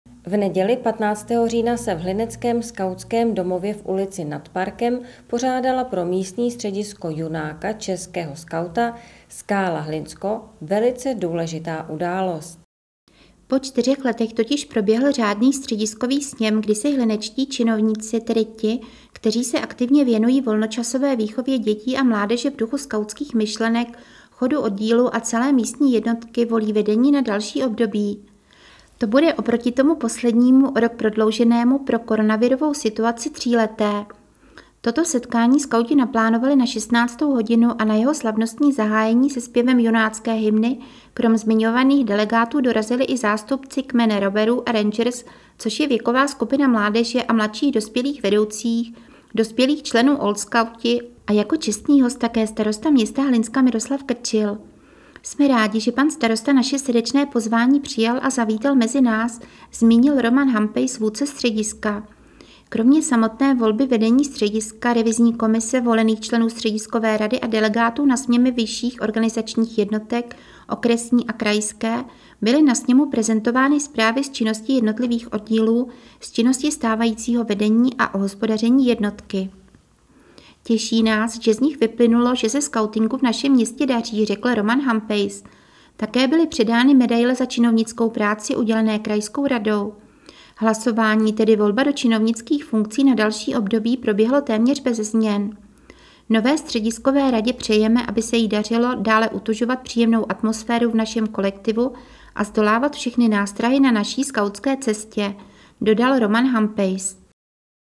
44/2023 Skautský střediskový sněm v Hlinsku
V neděli 15. října se v hlineckém Skautském domově v ulici Nad Parkem pořádala pro místní středisko Junáka - českého skauta, Skála Hlinsko, velice důležitá událost.